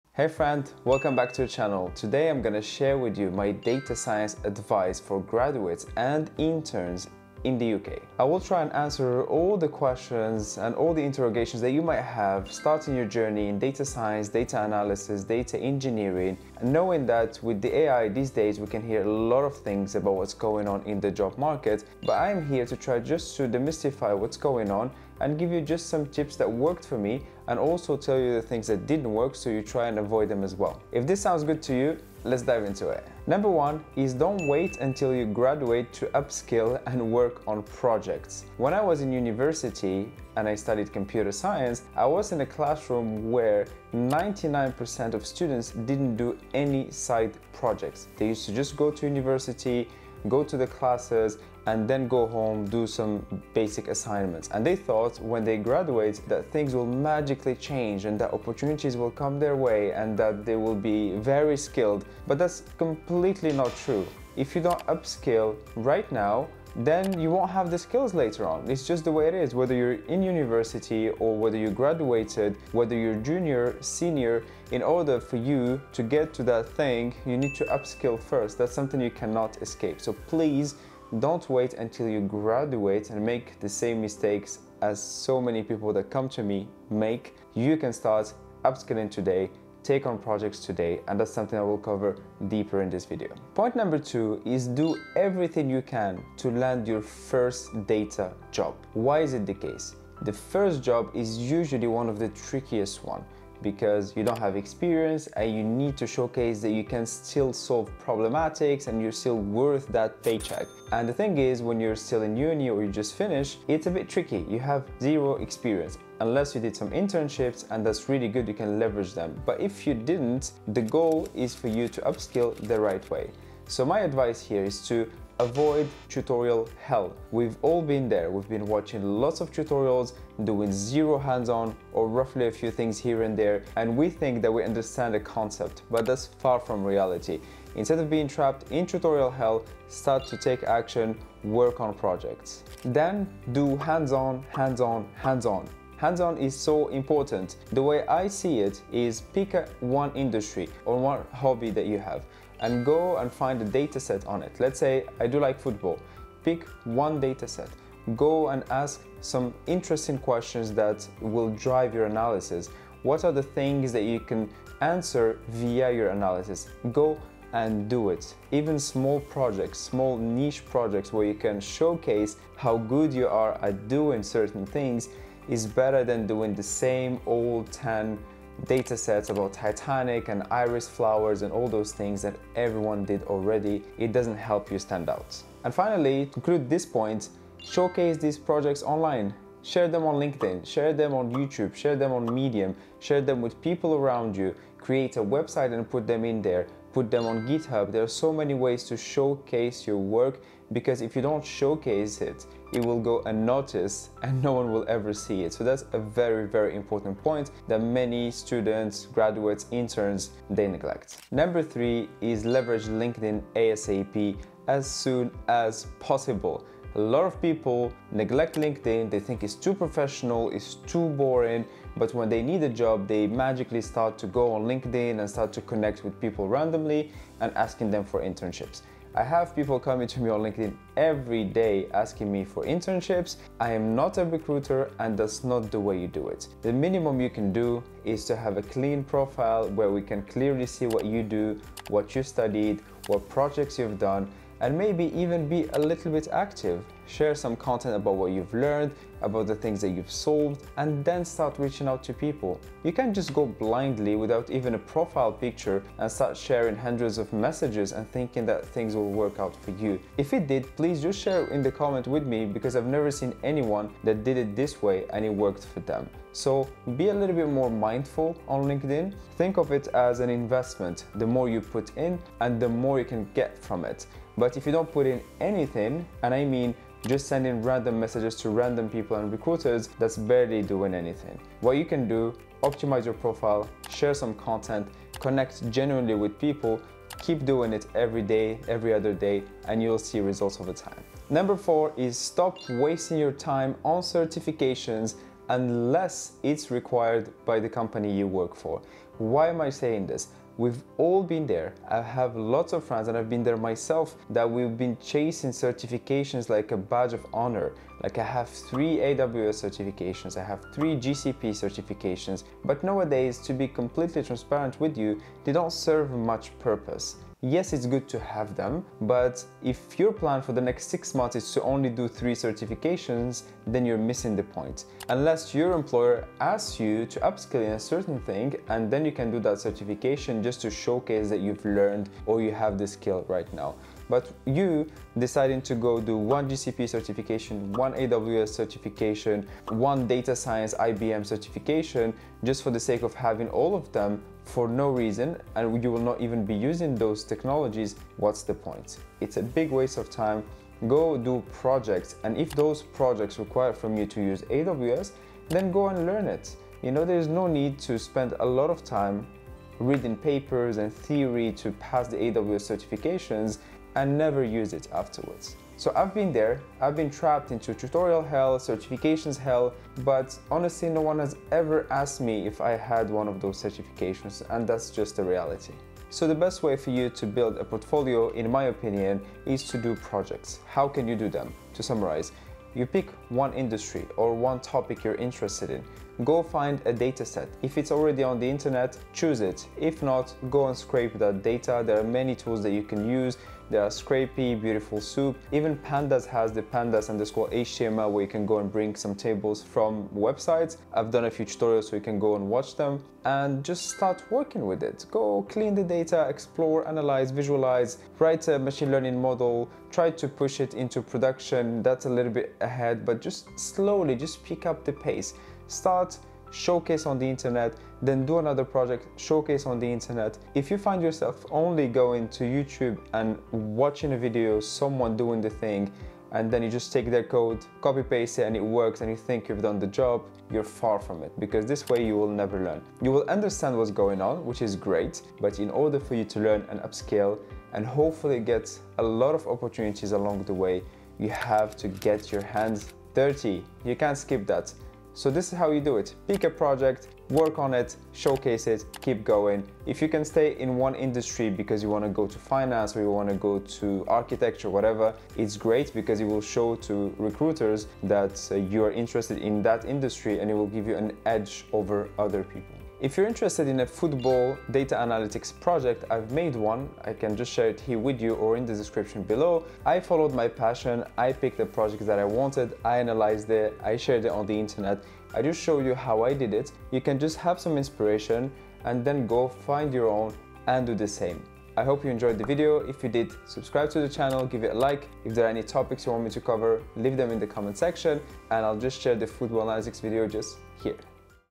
Data Science Tips for UK Graduates & Interns (2025 Guide)
Getting a data science job in the UK isn't about collecting certificates or watching hours of tutorials. It's about getting hands-on, building real projects, and showing your work.

In this video, I break down exactly how grads and interns in the UK can land their first data role in 2025 without wasting time.

🎯 Here’s what you’ll learn:

- Why most students stay stuck in tutorial hell
- How to break out with simple, focused projects
- What to do instead of chasing random certificates
- How to use LinkedIn to get real job leads
- The easiest way to build your first end-to-end project

🔍 Tips inside this video:
✅ Pick one topic and start with a small idea
✅ Set a basic goal and search for data
✅ Build a project—even if it’s messy
✅ Share your work online (especially on LinkedIn)
✅ Avoid wasting time on fancy courses unless your job requires it
✅ Why hands-on learning is always better than watching others code

👥 This video is perfect for:

- UK students in data science, computer science, or analytics
- Fresh grads trying to land their first job
- Interns looking to turn experience into full-time offers
- Anyone tired of tutorials and ready to start doing

💬 Drop a comment with your next project idea!

TIMESTAMP:
0:00 - Intro
0:33 - Tip 1
1:33 - Tip 2
3:31 - Tip 3
4:51 - Tip 4
6:20 - Tip 5